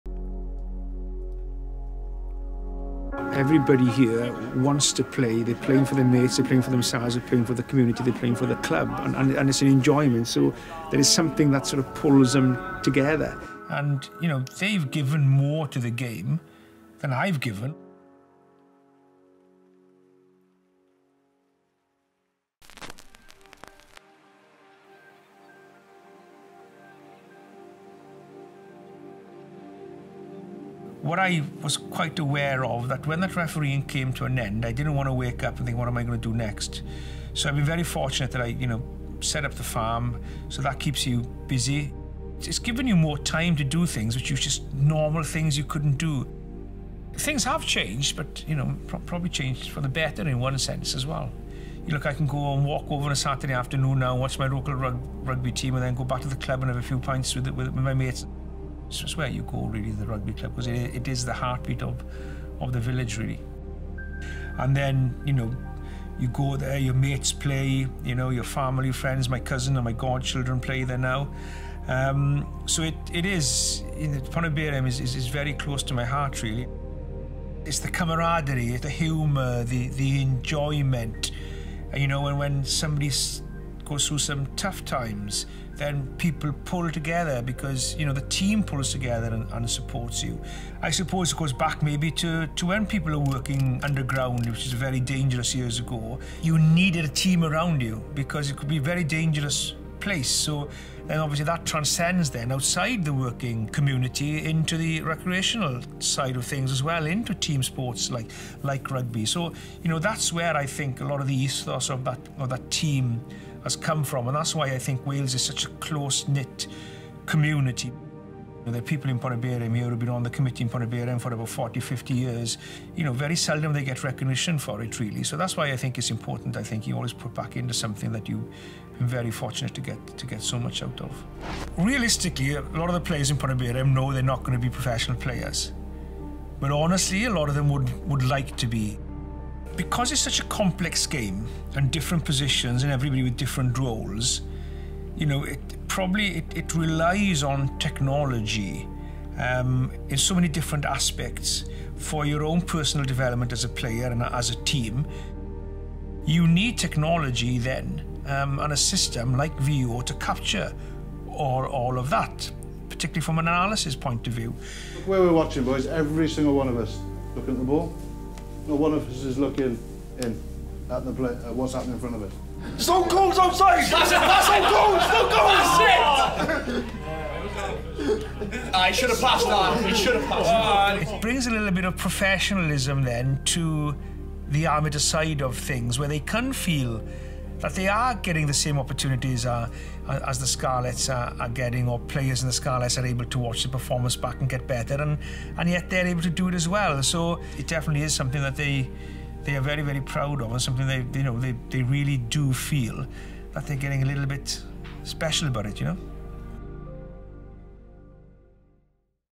Nigel Owens - For the love of the game
Join Rugby royalty Nigel Owens as he takes Veo back to his hometown and life outside of professional Rugby  - from the world's biggest stage to his local club Pontyberem RFC.

Nigel reflects on his career as an international referee and how important rugby is for communities at local club level. Often considered the ‘beating heart’ of a town, the significance of the local rugby club is huge. Nigel talks about the impact technology like Veo is having back at his old club Pontyberem, giving them as close to a professional experience as possible - rejuvenating this small rugby club.

Shooter Director - Fin Frew
AC & 2nd Cam Op - Sam Simmons-Betts
Sound Op - Louis Tompkins